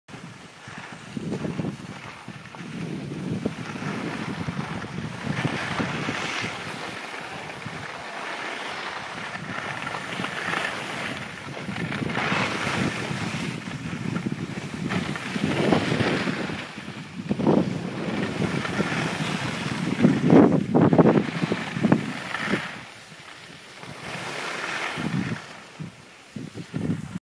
Isabella Skiing
Skiing from Timberline to Center Village. Big girl stuff!!  Look at those turns!